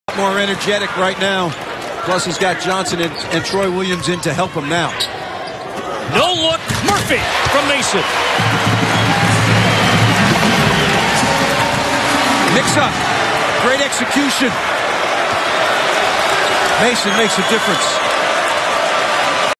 Nate Mason No-Look Pass to Jordan Murphy vs. Indiana
Minnesota's Nate Mason makes a no-look pass to Jordan Murphy for the slam dunk against Indiana in the second half.